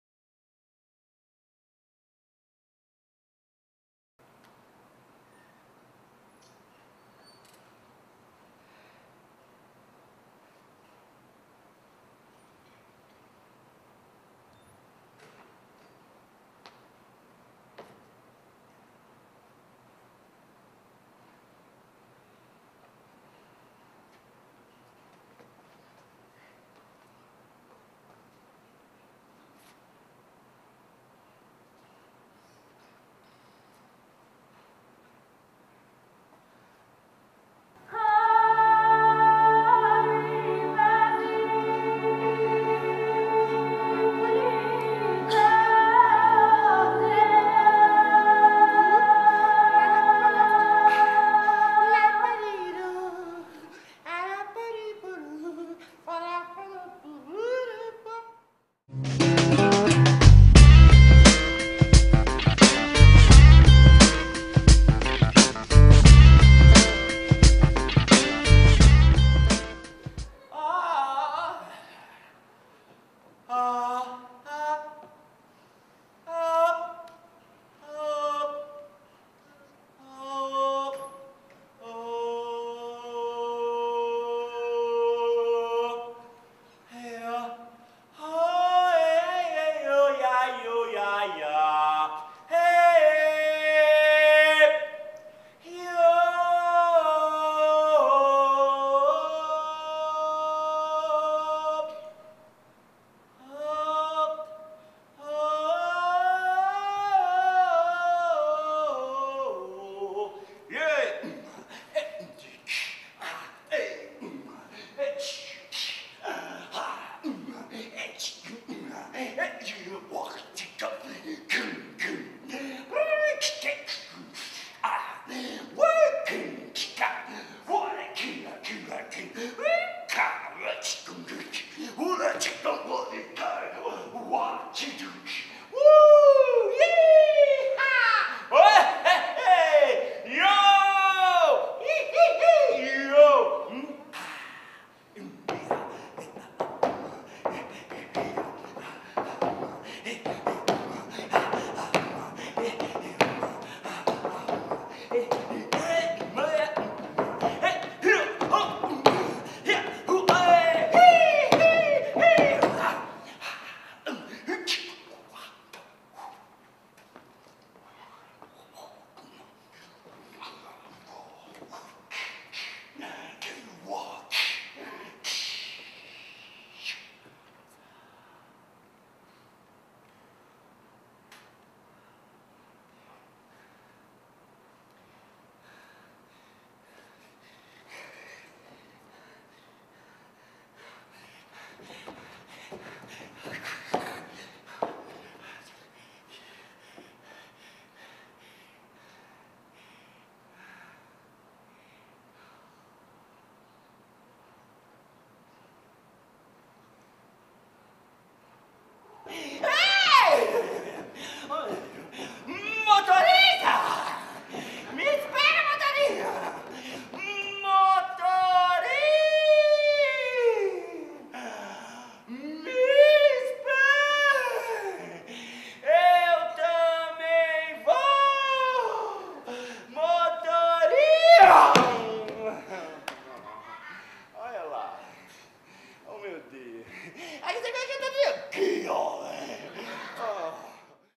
2ª Mostra Internacional de Mímica_17_Miqueias Paz_MIMICANDO 30 ANOS_SESI Taguatinga
MATERIA PRIMMA e FAC apresentam II Mostra Internacional de Mímica, de 17 a 26 de maio de 2013, Teatro Funarte Plínio Marcos & SESI Taguatinga. O vídeo a seguir é um mix com trechos da apresentação e possui a duração de aproximadamente 5 minutos.
Miqueias Paz(Brasil/DF), espetáculo MIMICANDO 30 ANOS, 26 de maio de 2013, 20h, Taguatinga SESI Teatro Yara Amaral, Distrito Federal - Brasil. Realização do registro audiovisual Vinicius Borba Comunicação e Produção Cultural & Vídeo Master/Aurélio Pereira, contatos:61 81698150, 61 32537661, respectivamente.